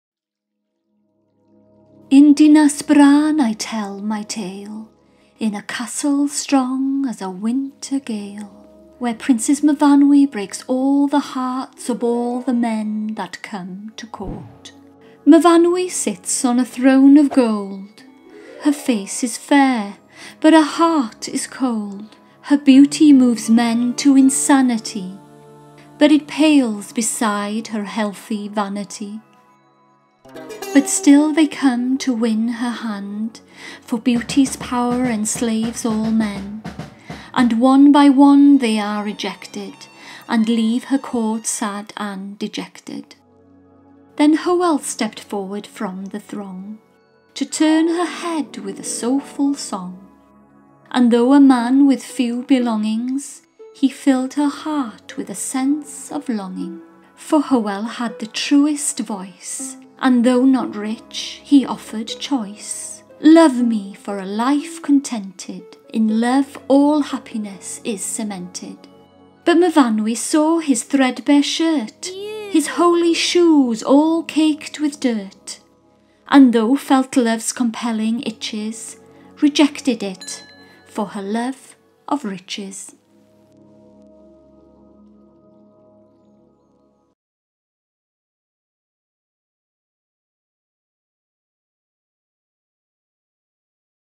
Myth Fest Wales - THE TALE OF HYWEL & MYFANWY
A 3 hour magical family adventure. A story of love lost at Castell Dinas Bran, but will their paths cross in 2018?